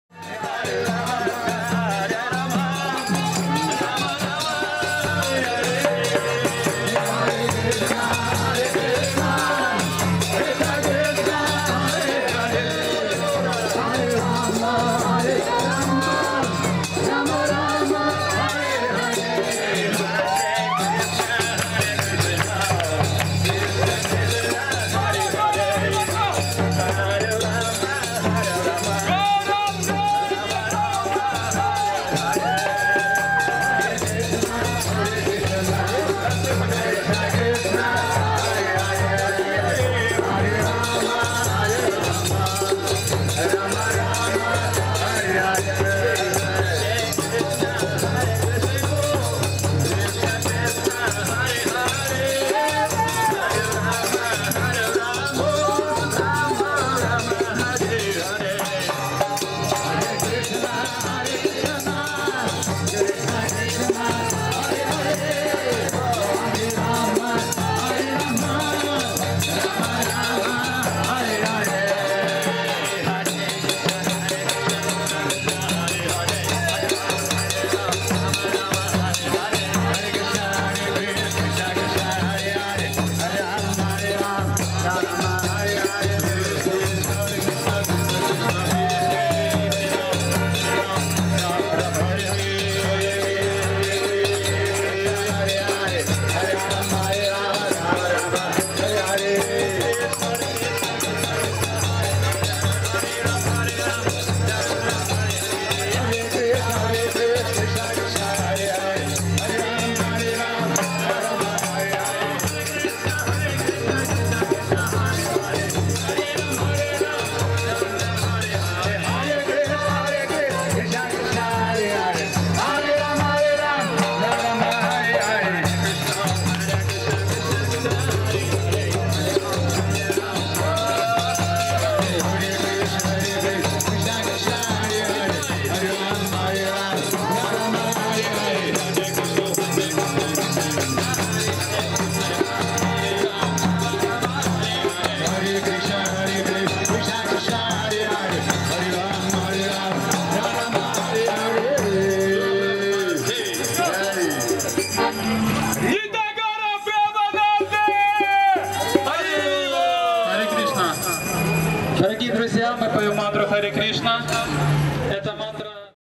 Фрагмент из жизни - Дух Падаятры в сердце Харькова (Харьков, 24 Августа 2020 г.)
..что тут скажешь? Дух Падаятры в самом сердце Харькова!